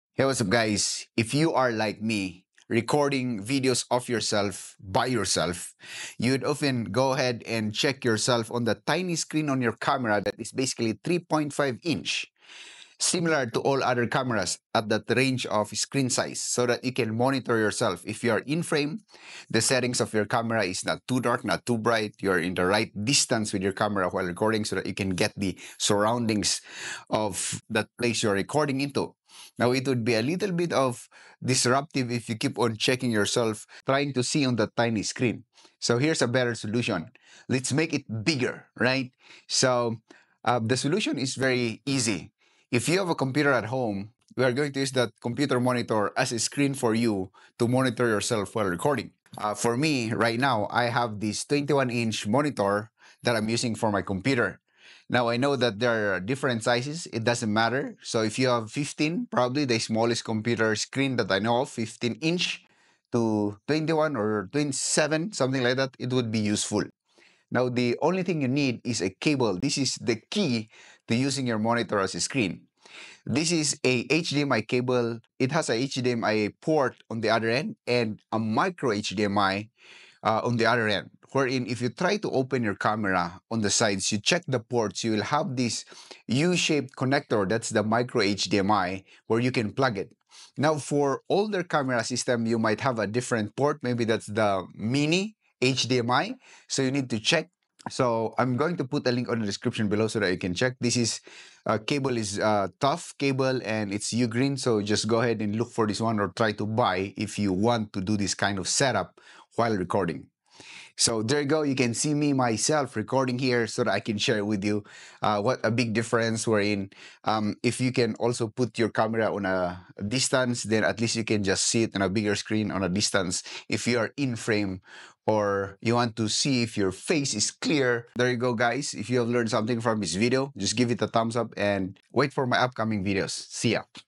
How I Use My Computer Screen as a Live Monitor When Filming Myself
🎥 Filming yourself alone can be tricky – staying in frame, checking focus, and ensuring the right lighting is a challenge without a camera operator. In this video, I show you how I use my computer screen as a live monitor to make solo recording easier and more professional.

HDMI CABLE:
HDMI and Micro HDMI